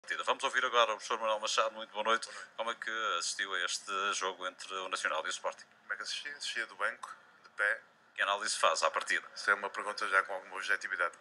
Manuel Machado - flash interview - Nacional vs Sporting
Nacional vs Sporting - Primeira Liga
Direitos pertencentes à Sporttv